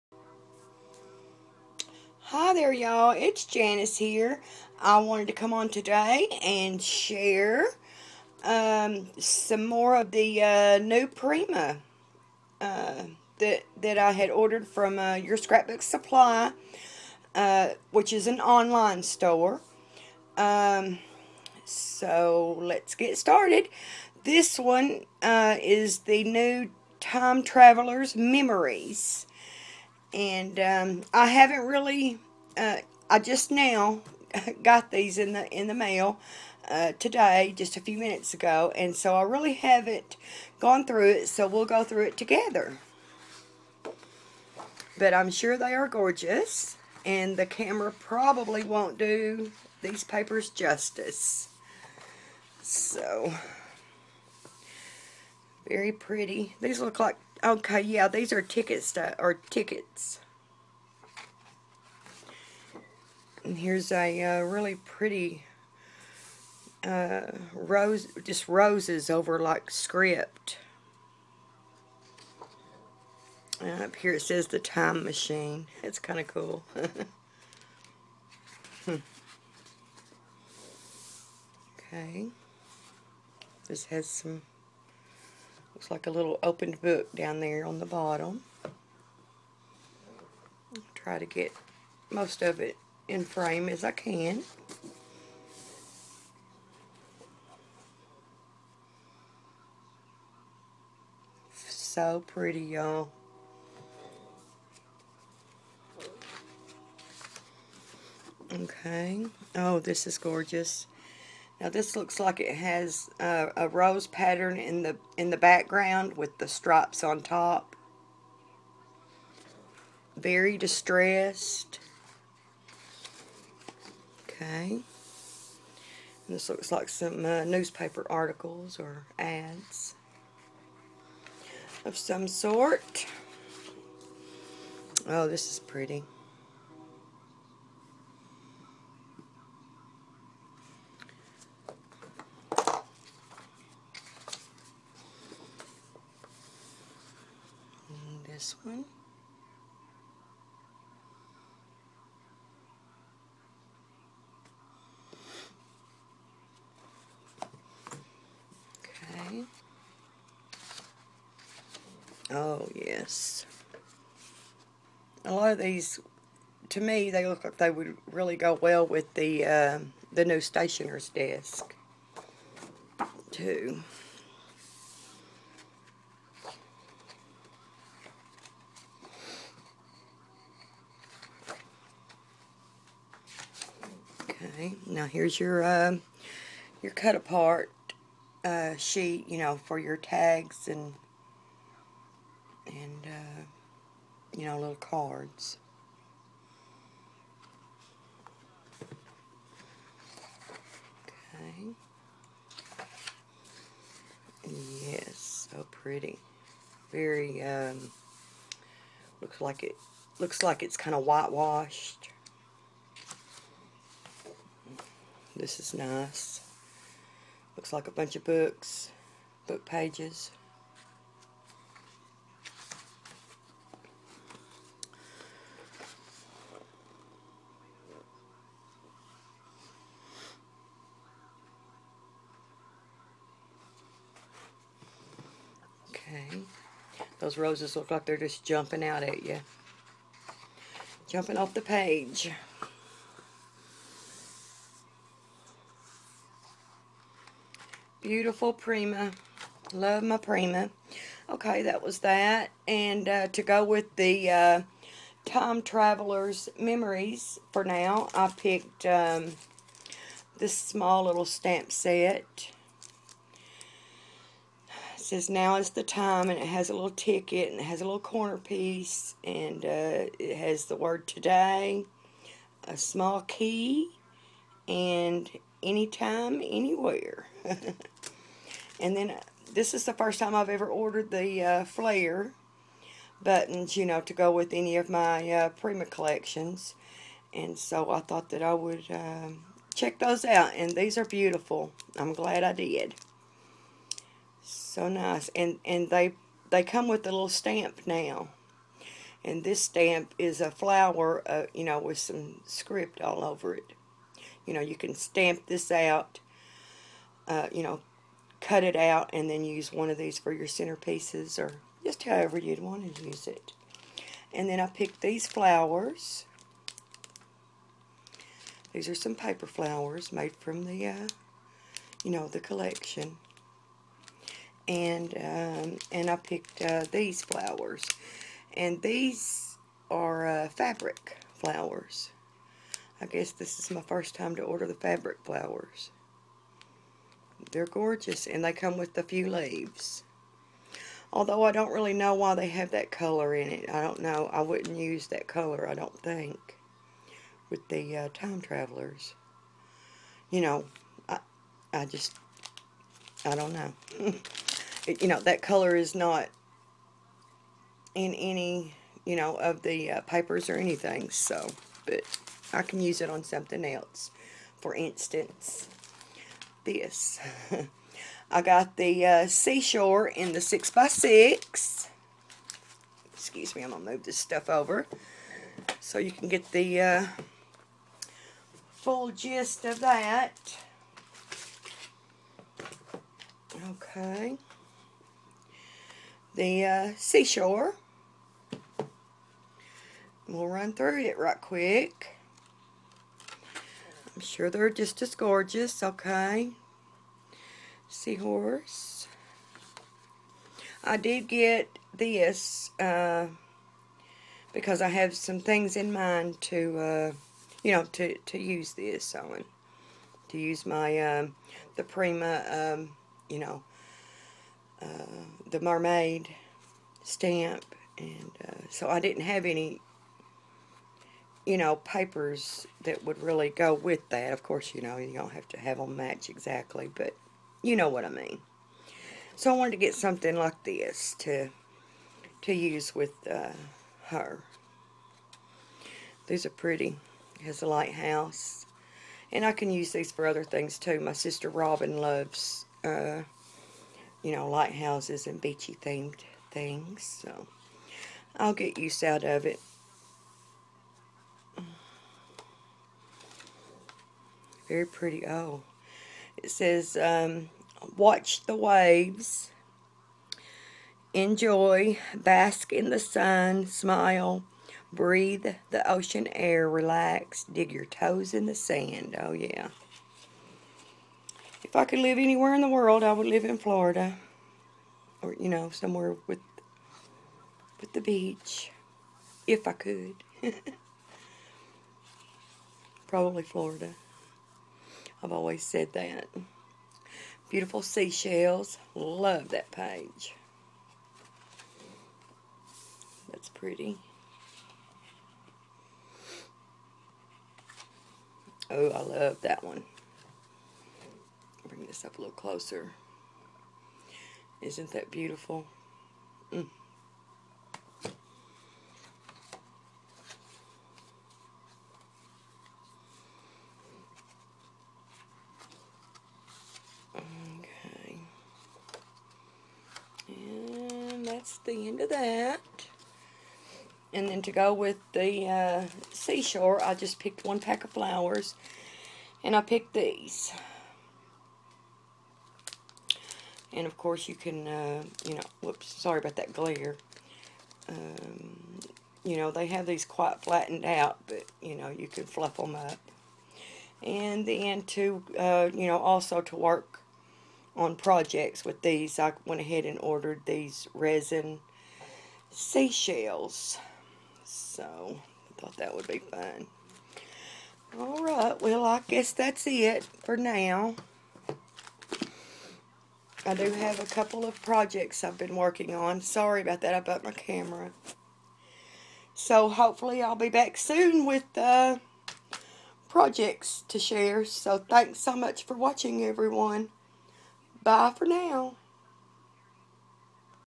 New Prima Haul
These are the 2 Prima Collections just released for 2014, Time Travelers Memories & Seashore.